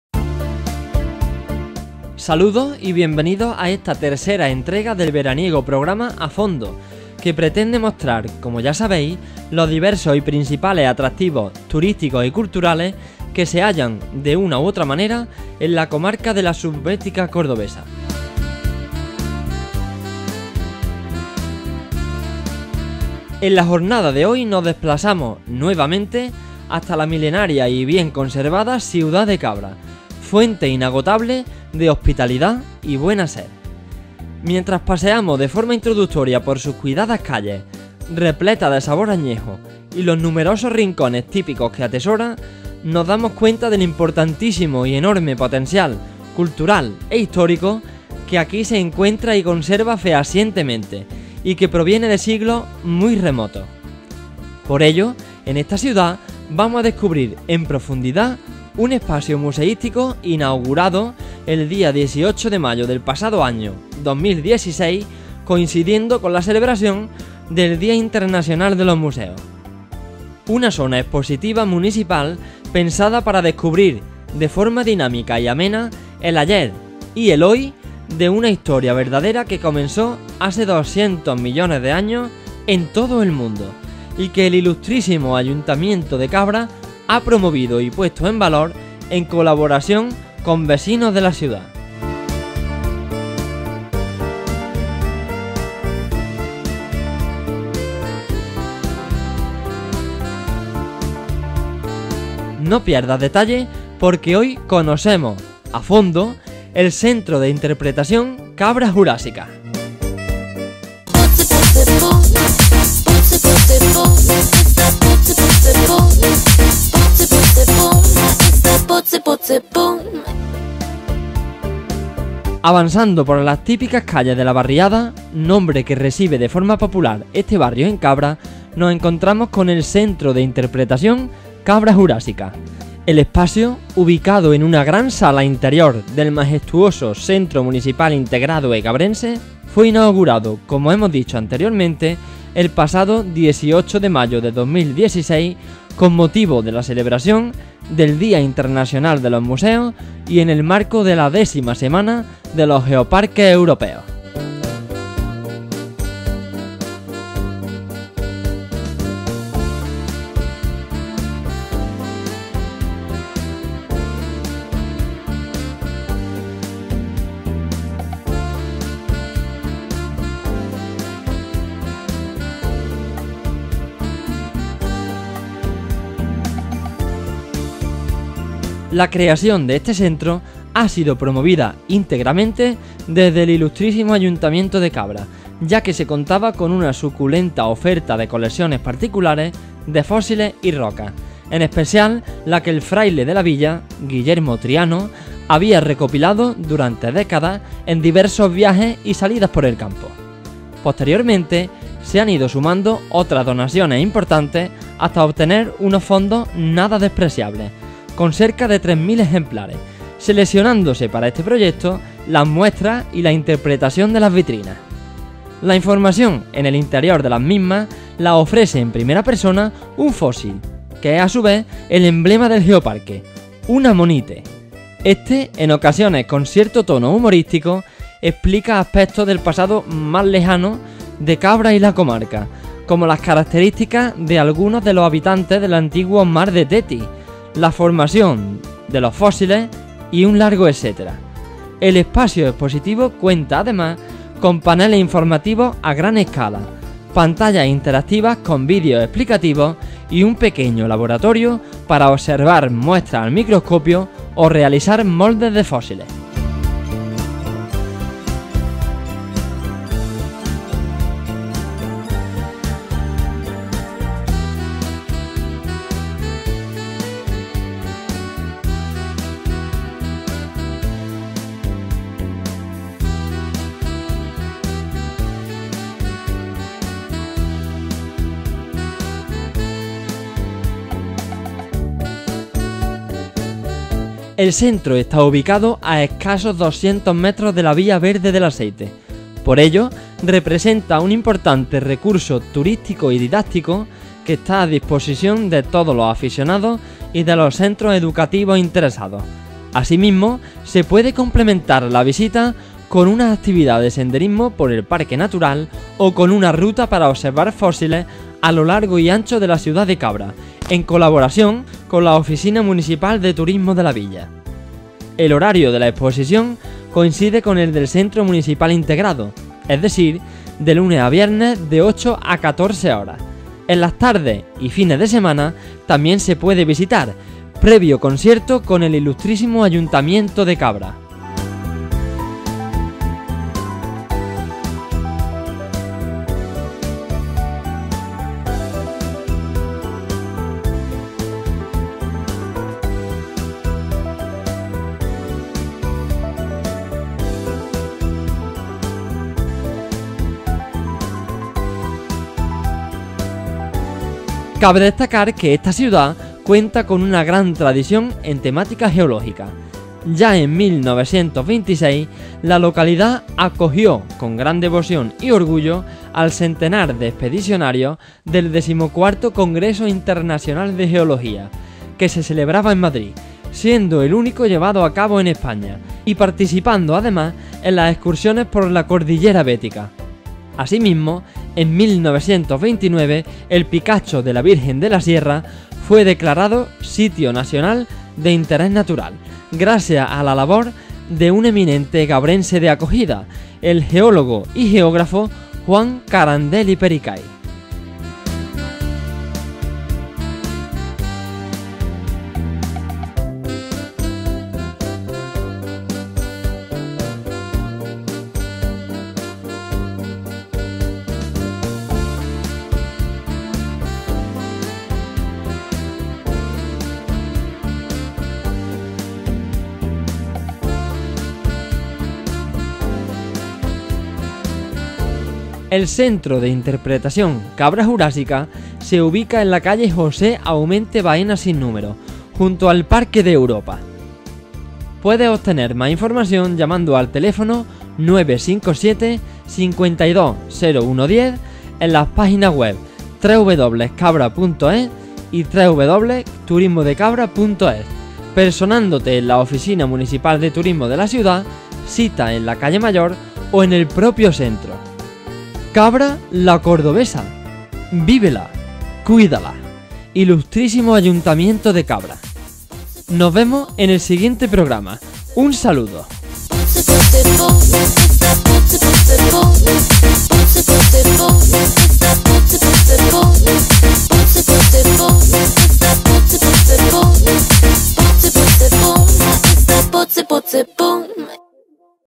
A fondo. Programa III. C.I. Cabra Jurásica. Cabra (Córdoba)
Saludos y bienvenidos a esta tercera entrega del veraniego programa ‘A fondo’, que pretende mostrar, como ya sabéis, los diversos y principales atractivos turísticos y culturales que se hallan, de una u otra manera, en la comarca de la subbética cordobesa. En la jornada de hoy nos desplazamos, nuevamente, hasta la milenaria y bien conservada ciudad de Cabra, fuente de hospitalidad y buen hacer. Mientras paseamos de forma introductoria por sus cuidadas calles, repletas de sabor añejo, y los numerosos rincones típicos que atesora, nos damos cuenta del enorme potencial cultural e histórico que aquí se encuentra y conserva fehacientemente, y que proviene de siglos muy remotos.

Por ello, en esta ciudad vamos a descubrir en profundidad un espacio museístico inaugurado el día 18 de mayo del pasado año, 2016, coincidiendo con la celebración del día internacional de los museos. Una zona expositiva municipal pensada para descubrir, de forma dinámica y amena, el ayer y el hoy de una historia verdadera que comenzó hace 200 millones de años en todo el mundo, y que el Ilustrísimo Ayuntamiento de Cabra ha promovido y puesto en valor en colaboración con vecinos de la ciudad.

No pierdas detalle porque hoy conocemos, a fondo, el centro de interpretación Cabra Jurásica.

Cabra, la cordobesa. ¡Vívela! ¡Cuídala!

Nota: los comentarios serán revisados uno a uno y, si procede, publicados a la mayor brevedad posible. Disculpe las molestias.

- Canal de YouTube: TJenlared Rute